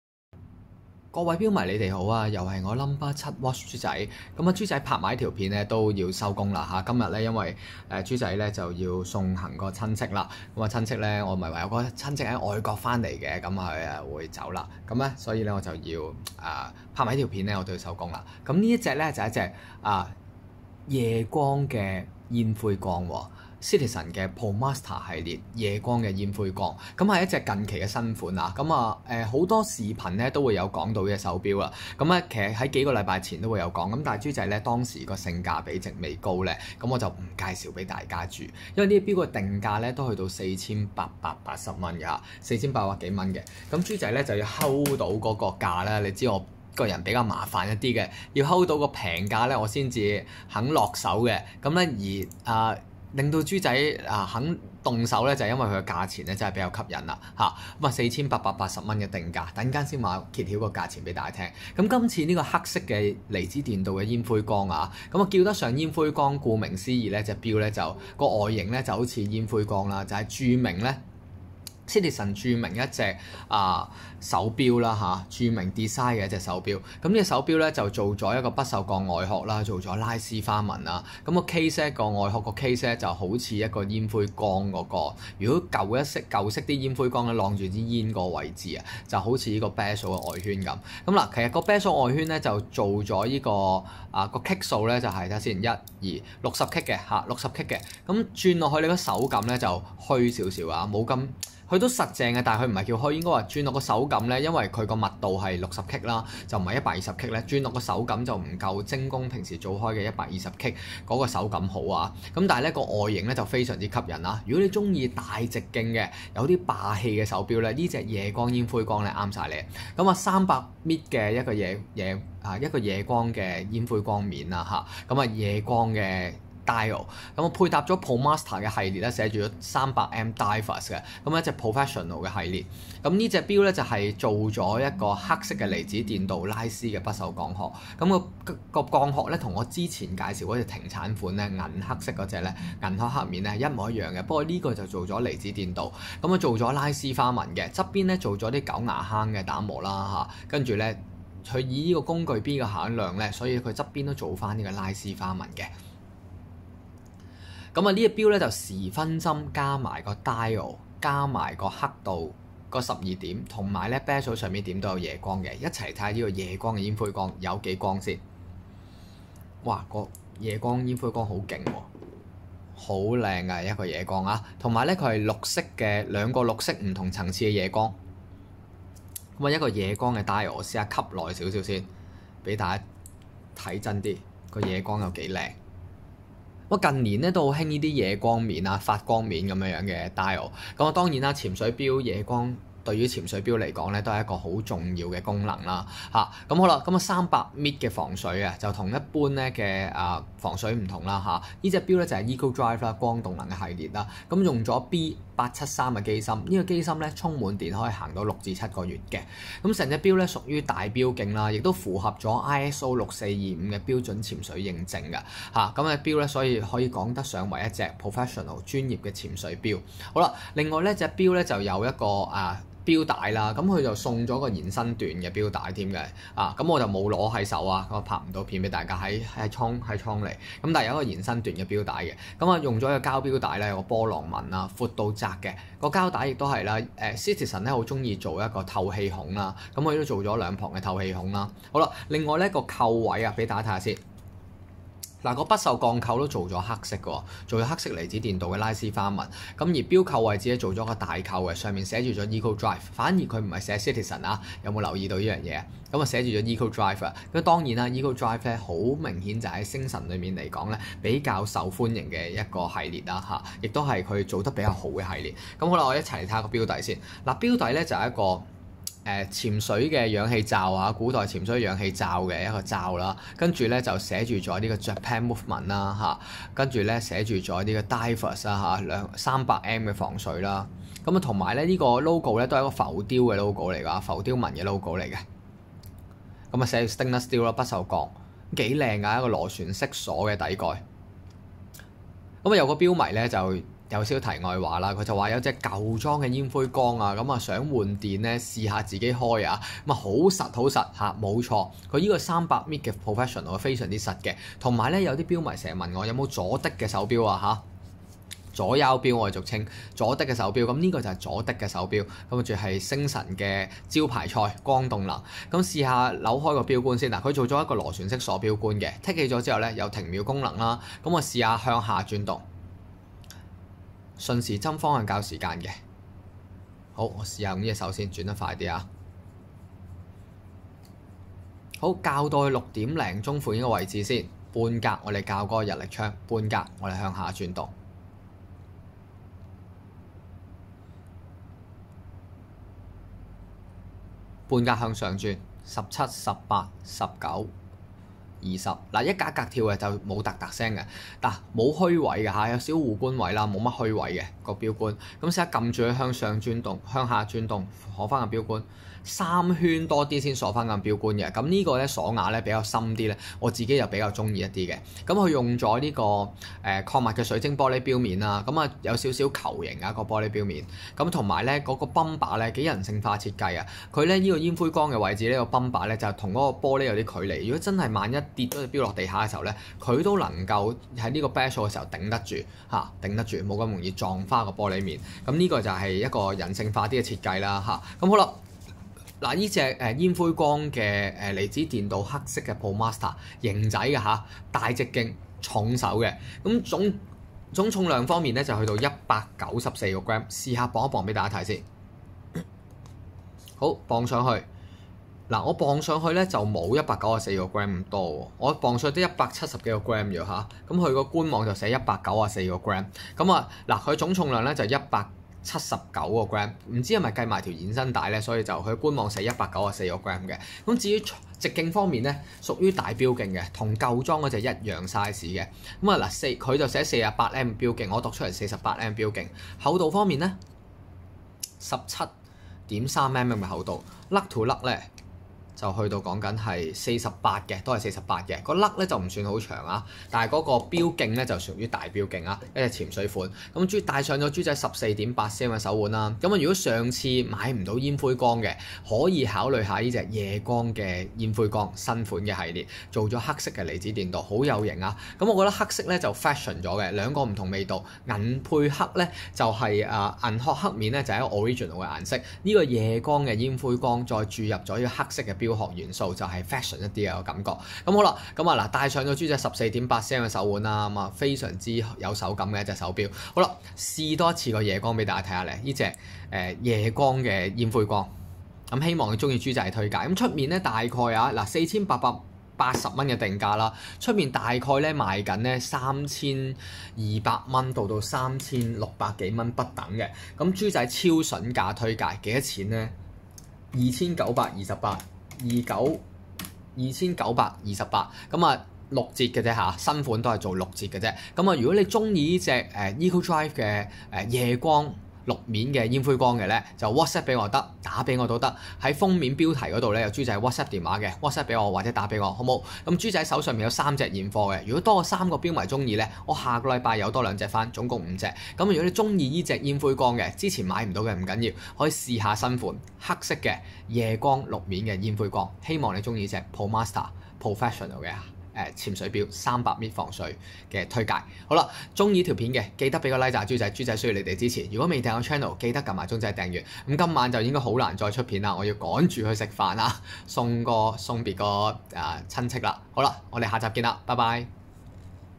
CITIZEN Promaster Eco Drive 朱仔推介平玩《夜光「煙灰缸」光能🌞專業潛水錶》Hk2928😳🌊300m tel 96699429 🎉
CITIZEN Promaster 夜光「煙灰缸」

機芯 B873
充滿電力後
最長運轉時間
(正常使用) 約 6 個月
動力 光動能
訊號接收 沒有電波接收信號功能
錶帶 合成橡膠錶帶
防水 300米防水
重量 194g
厚度 18.5mm
錶徑 48.2mm

錶殼 不銹鋼
表面處理 黑色離子電鍍
錶面 水晶玻璃錶面

產品功能
符合ISO6425潛水錶標準
日期顯示
光動能 (以任何光源充電，毋須定期更換電池)
電池能量不足指示功能
防止過度充電功能
附有合成橡膠延長錶帶

有citizen盒說明書保用證及收據一年保用
荔枝角店或佐敦店交收有門市
現金價Hk2928🎖️🌊🌞🎉✨😳
有意請whatsapp或者致電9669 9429查詢
Welcome wholesale & retail